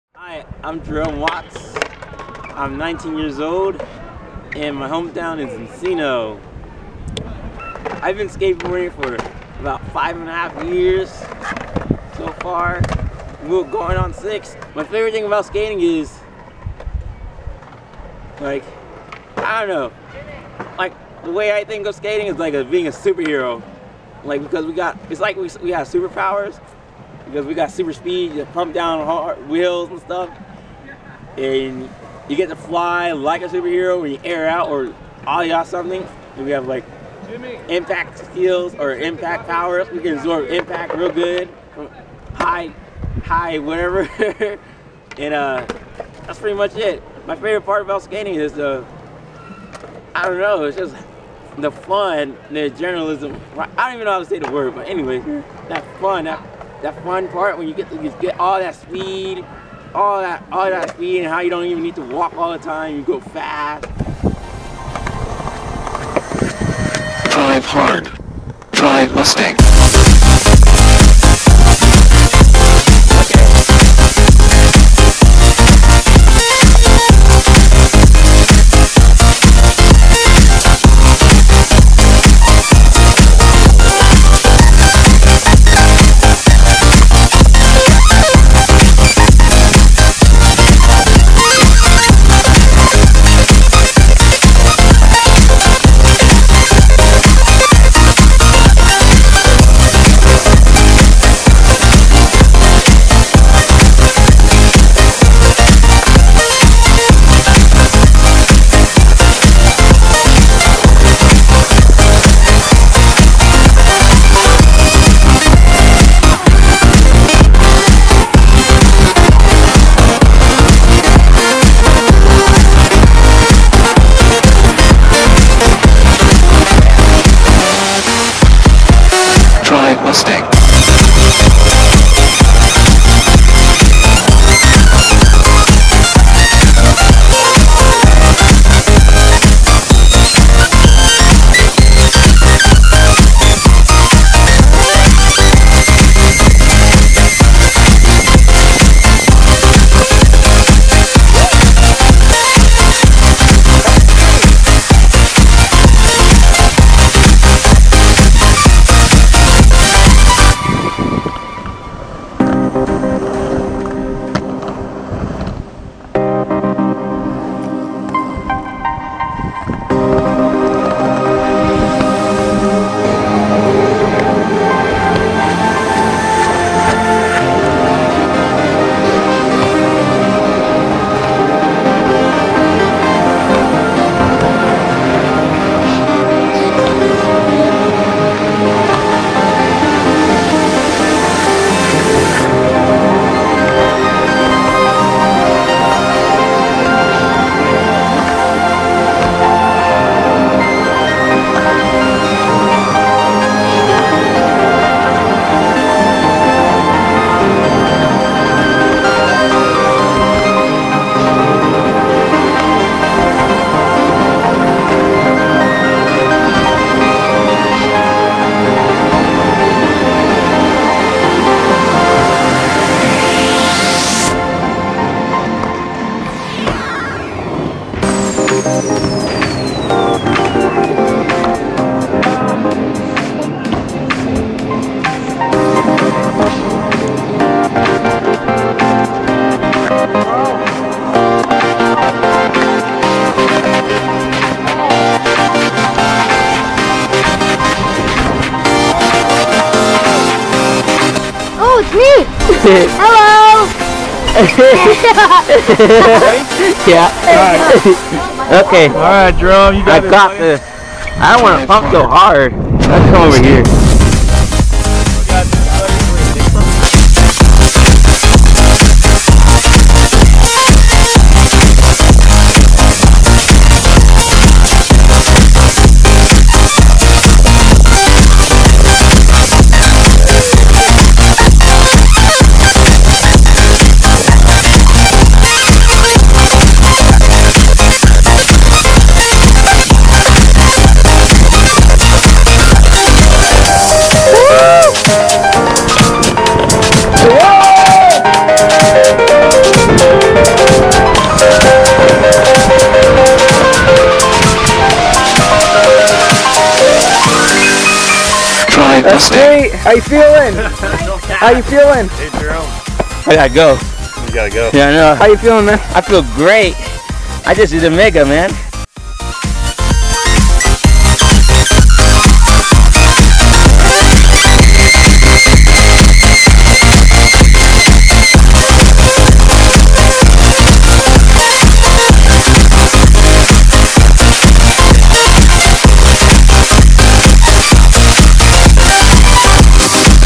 Jerome Video
This is Jerome Watts at Woodward West as a camper/worker. Sorry the audio is messed up because of youtube.